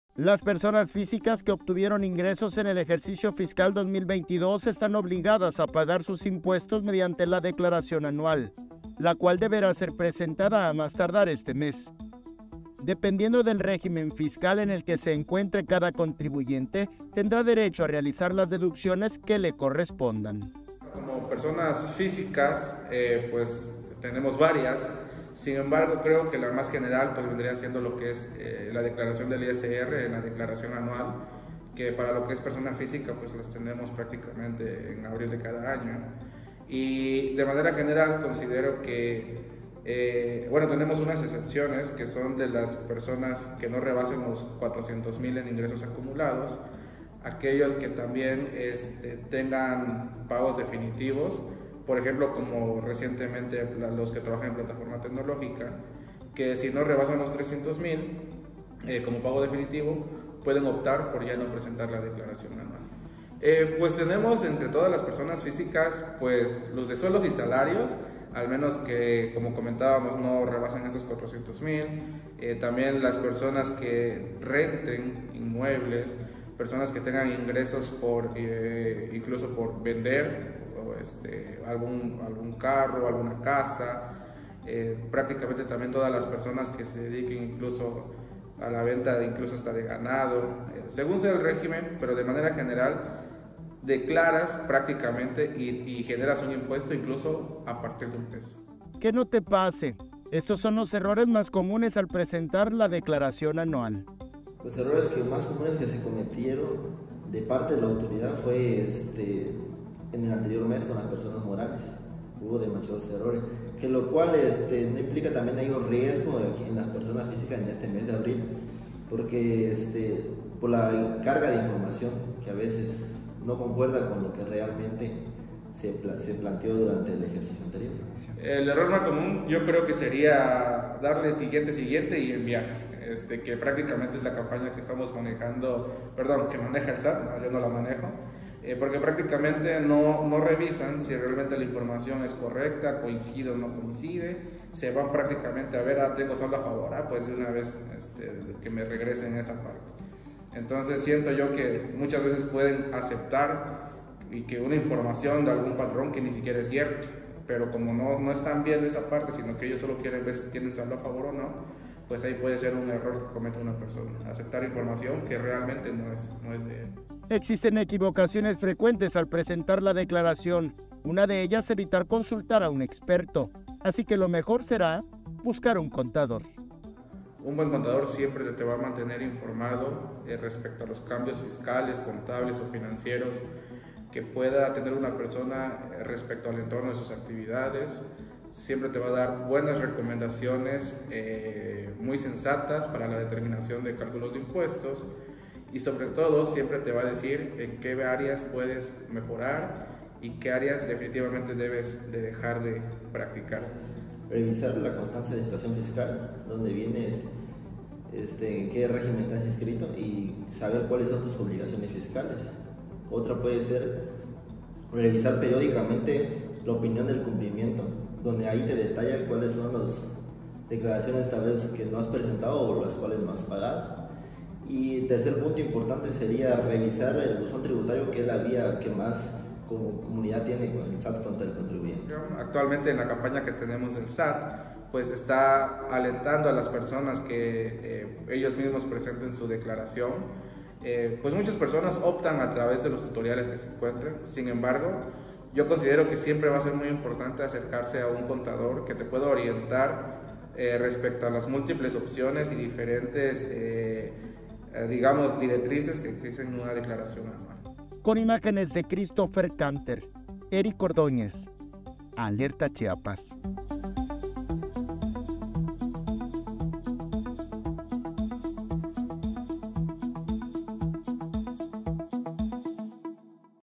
¿Eres Persona Física y tienes que presentar tu Declaración Anual?
Si obtuviste ingresos en el Ejercicio Fiscal 2022 están obligadas a pagar tus impuestos mediante la declaración anual.